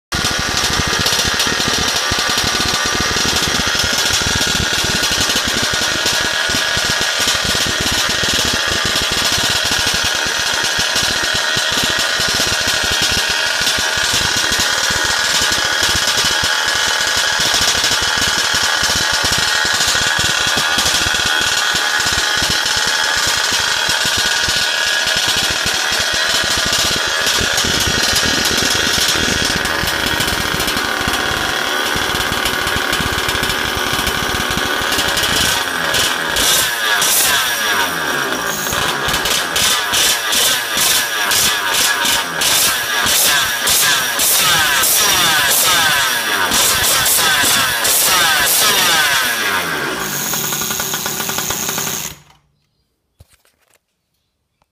First start up with the Flatto Racing SF  RC 40 pipe.